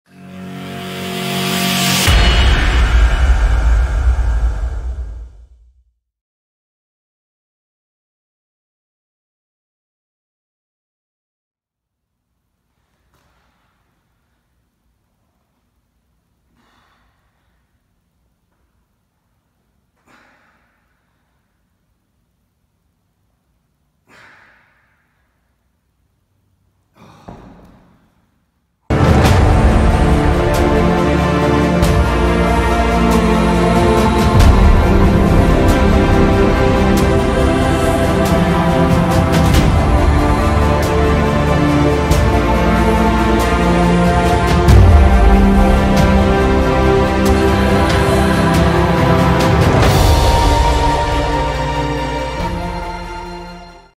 MB V Sit Puppet Choppers
Core drill with an emphasis on anti-extension and anti-rotation.